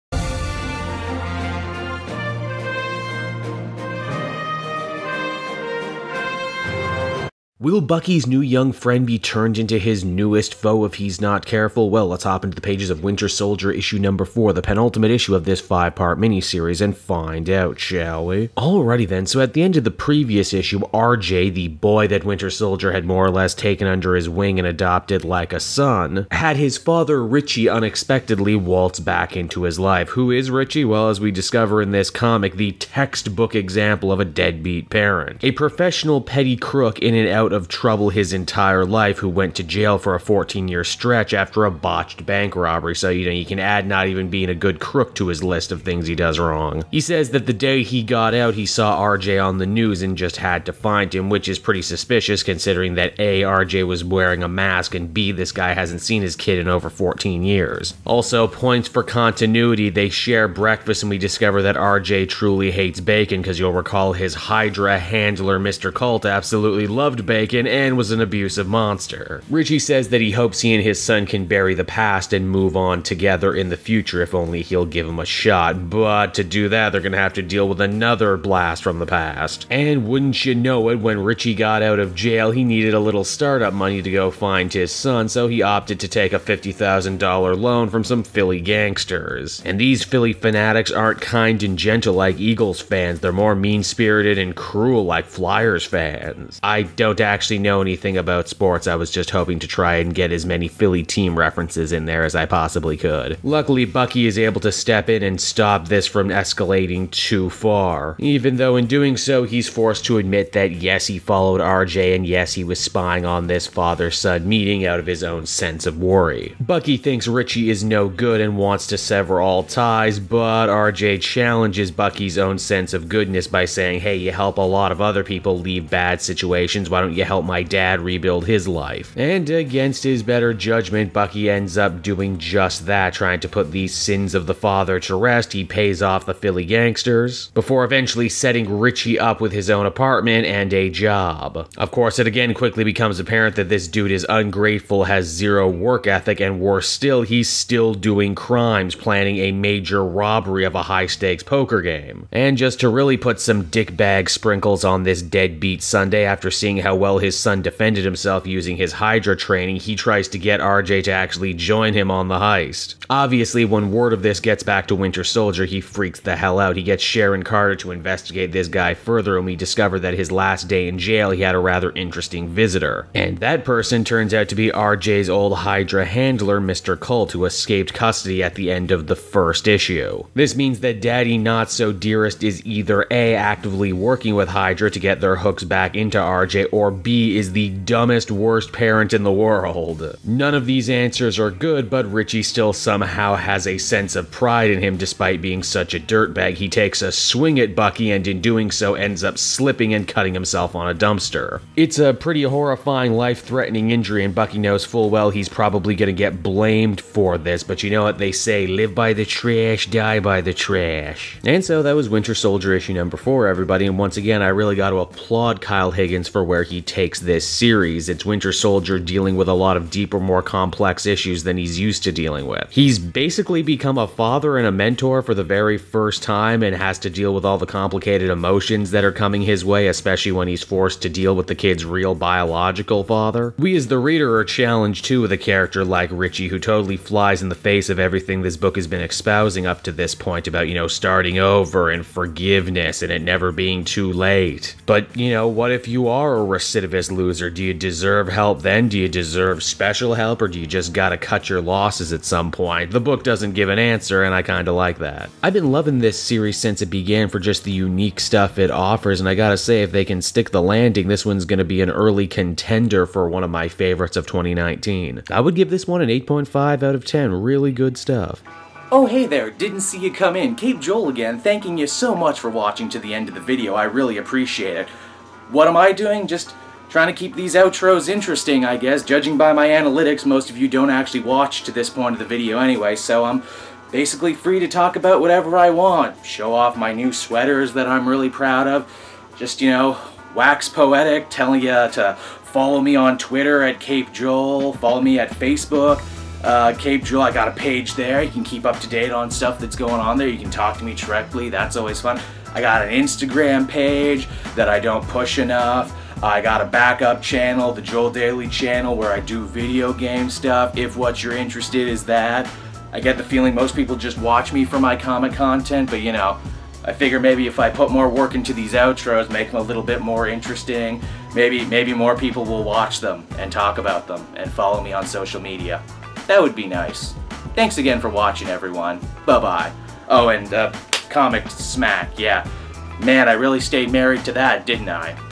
Sins Of The Father | Winter Soldier #4
Winter Soldier #4 - A face from the past returns at a pivotal time in RJ's deprogramming. But is his return too good to be true? Or is Bucky just being paranoid? What lengths is he willing to go to keep RJ safe? An issue filled with shocking choices.